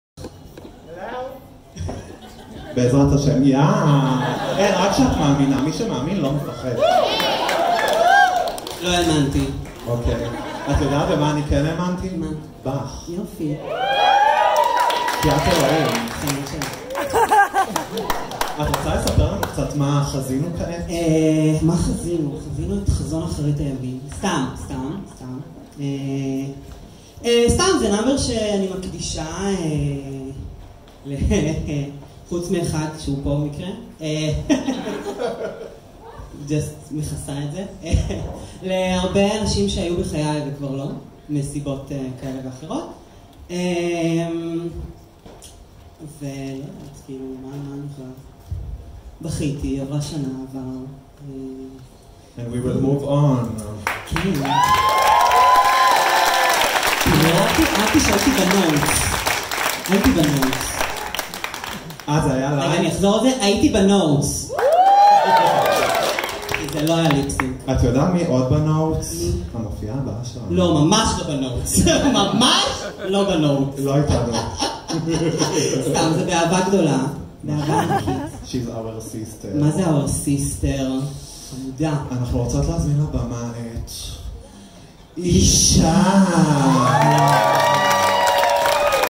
קרוס 26 - ג'ואנה ראס ומיס פלסטיקו מלרלרות
Joanna Russ & Miss Plastico

Live @ CROSS 26 - Farewell, Jacqueline!, Drag Show at Bascula, Tel Aviv 22.08.19